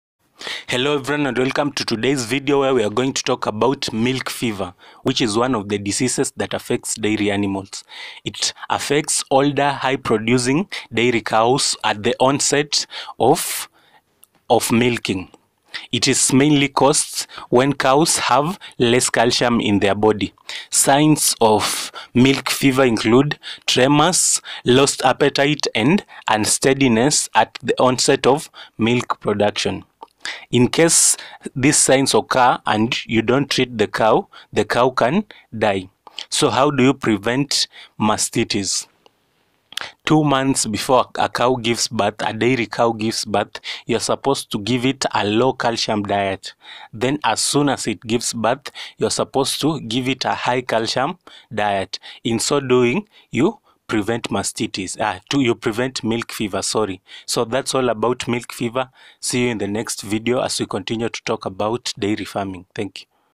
Milk fever disease in dairy animals | Can it kill your cow?
This video talks about Milk fever disease in dairy animals .
 It is my wish that this video in a small way, will help make you a better farmer.